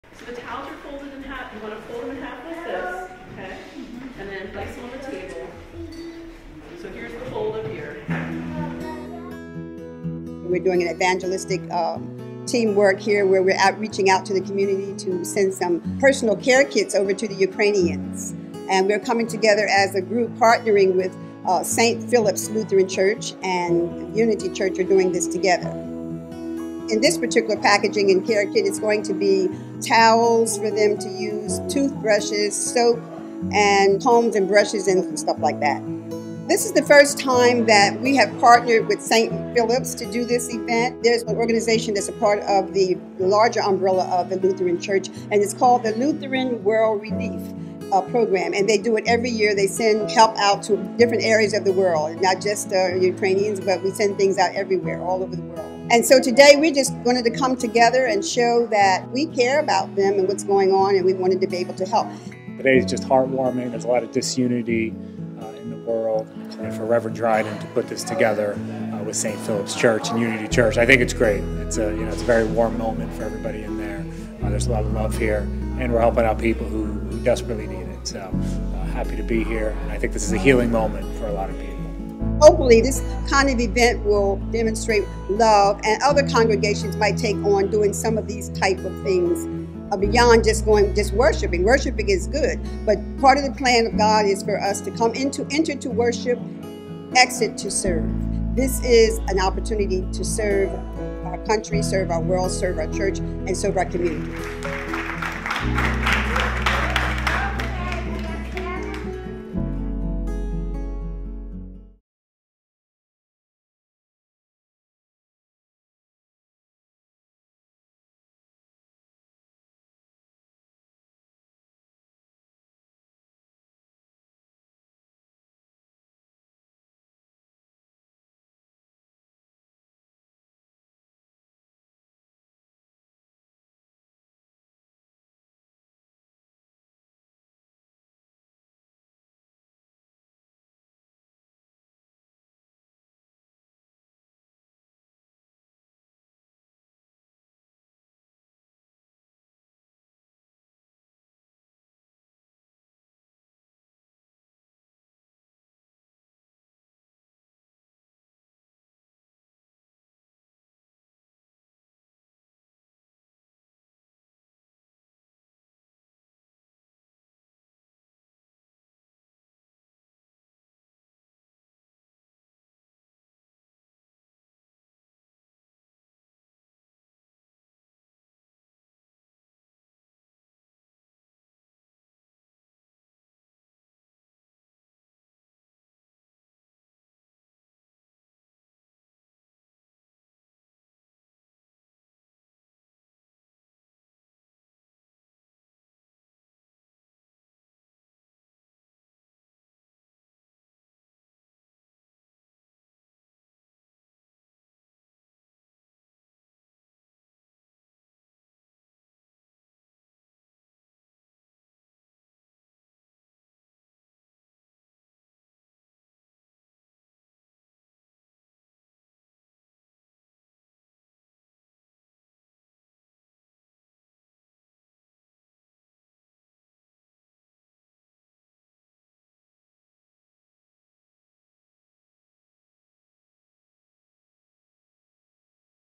22 News | Care Packages for Ukraine
Unity Lutheran Church and St. Phillips Lutheran Church partnered in an effort to put Personal Care Kits together for the people of Ukraine. The effort was held in cooperation with the National Lutheran World Relief Program.